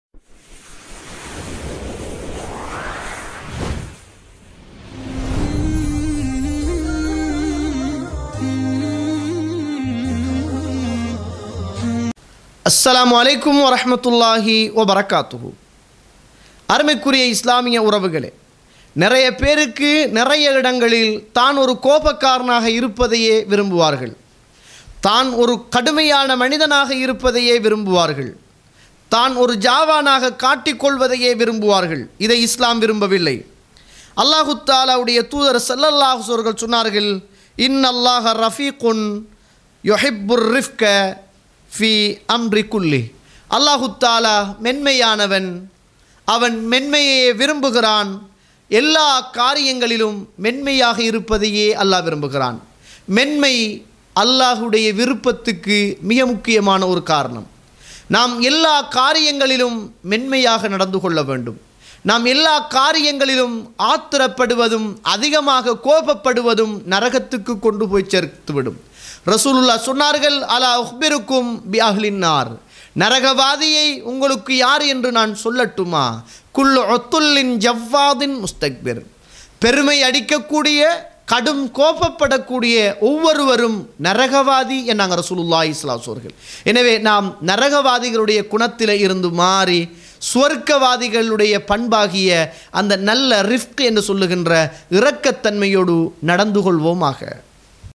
மேன்மை| தான் ஒரு கோபக்காரனாக இருப்பதை விரும்புகிறீர்களா
அது நரகத்துக்கு செல்பவர்களின் குணமாகும்
ரமழான் கற்றுத்தந்த பாடம்